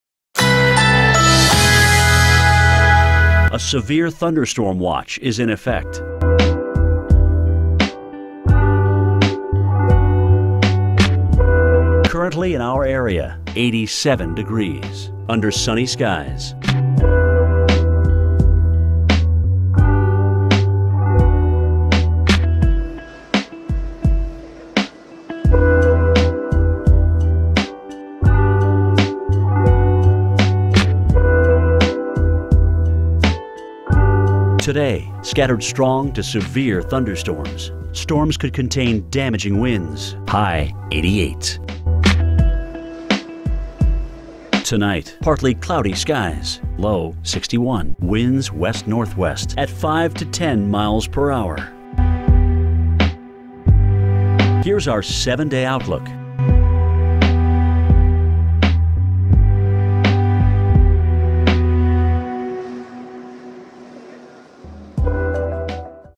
Local On The 8s Emulator - Fitchburg MA - 6/30/2024
Severe thunderstorm watches in effect due to strong to severe thunderstorms hitting the Fitchburg MA area. 60-70 mph winds and large hail are possible.

This emulation was made for: @Washing95